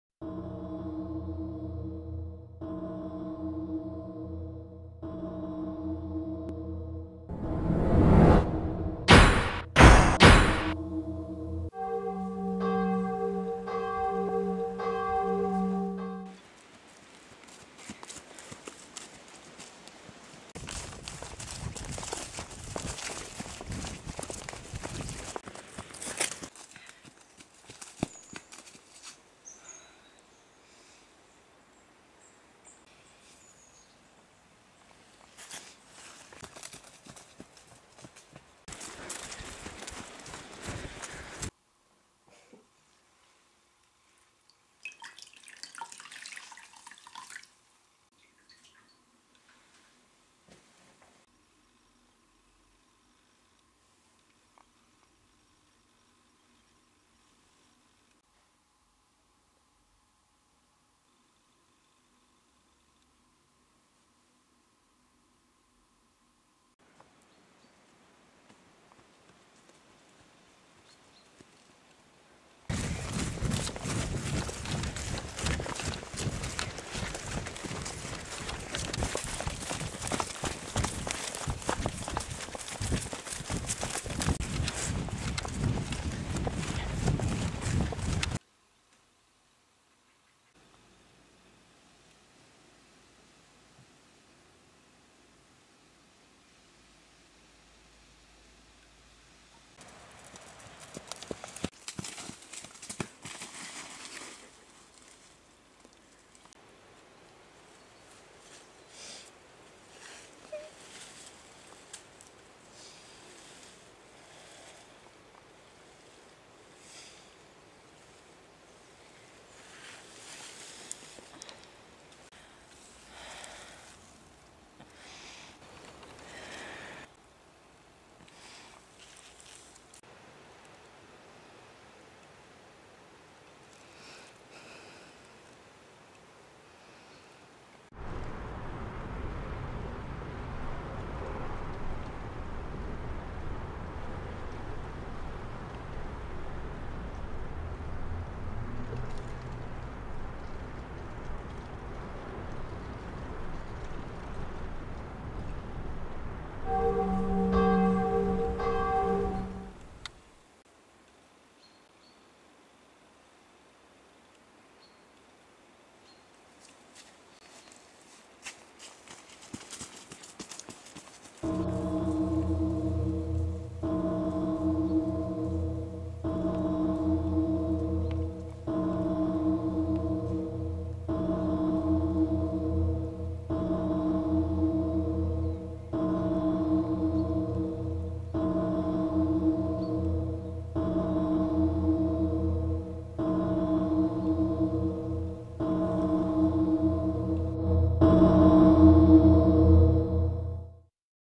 Flugten fra Livet
The film is about a girl, who is running away from her own life. She has some flashbacks from her life, which is about her dying sister.